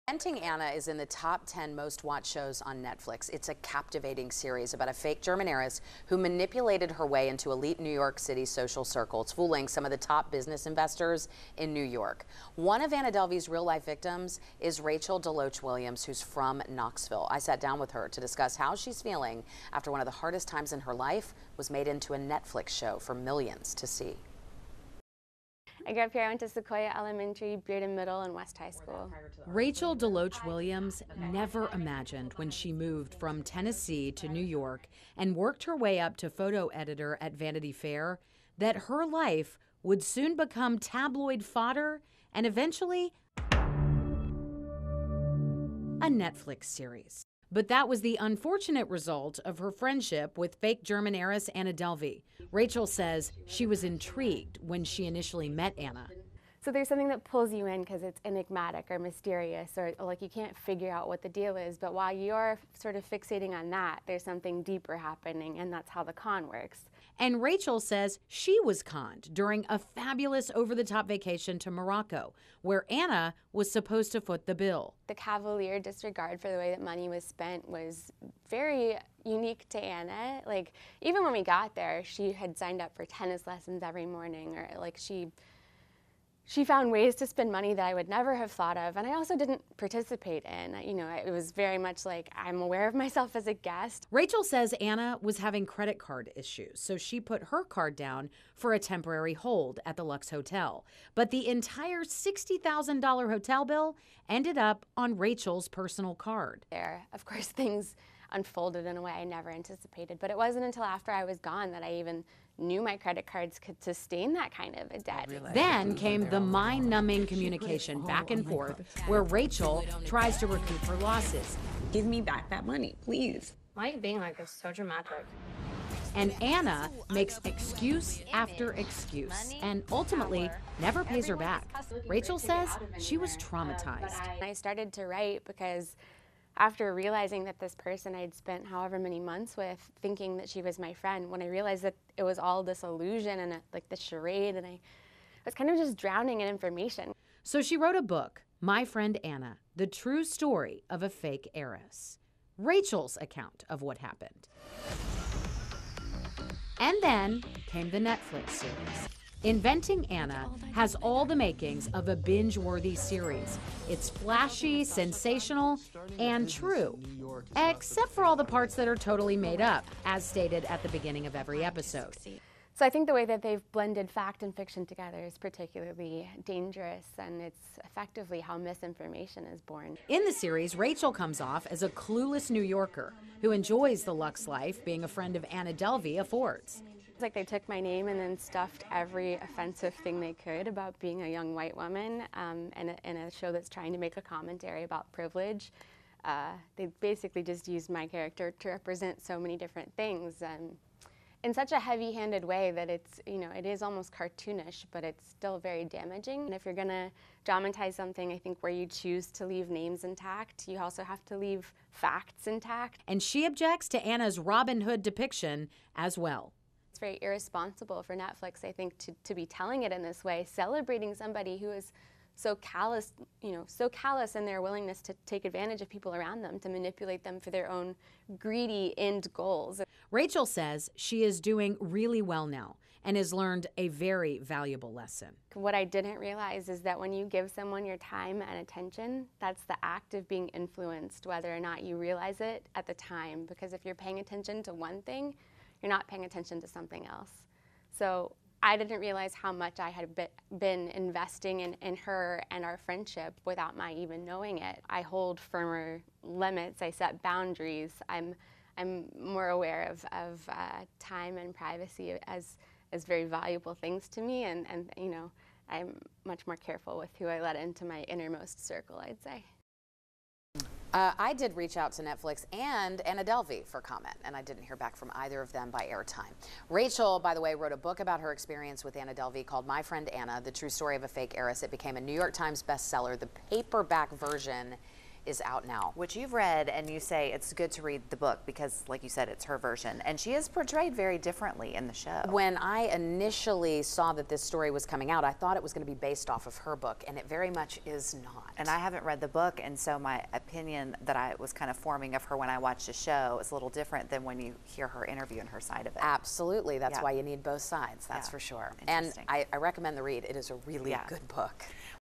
Rachel shares her real-life connections with Anna Delvey
Rachel Deloache Williams, a Knoxville native, shares her story about Anna Delvey, the con artist who is at the center of a new Netflix show, "Inventing Anna." Full story